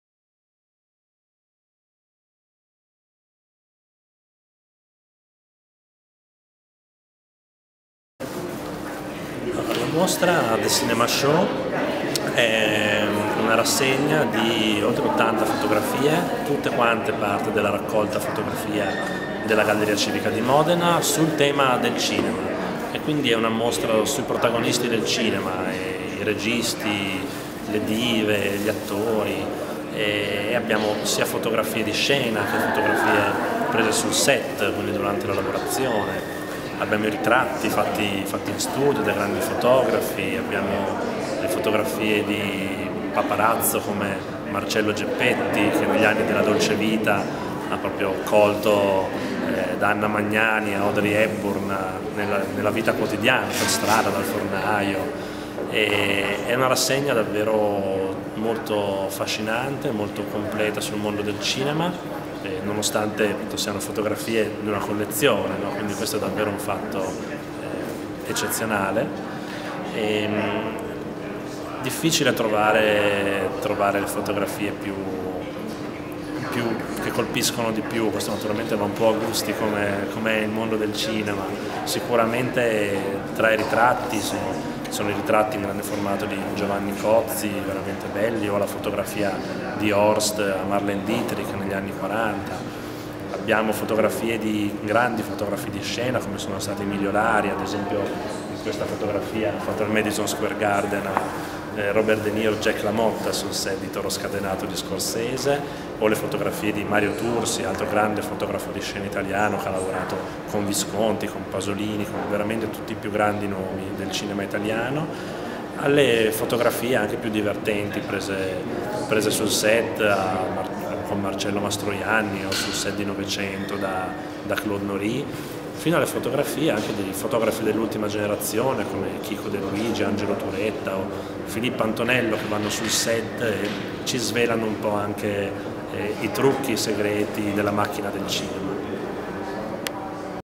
THE CINEMA SHOW
C'è quasi un secolo di cinema racchiuso nella mostra “The Cinema Show”, a Modena, a Palazzo Santa Margherita. Si tratta di un allestimento, a cura di Daniele De Luigi e Marco Pierini, con fotografie di scena, ritratti in studio, scatti eseguiti durante le pause sul set oppure scatti rubati in strada, di sorpresa.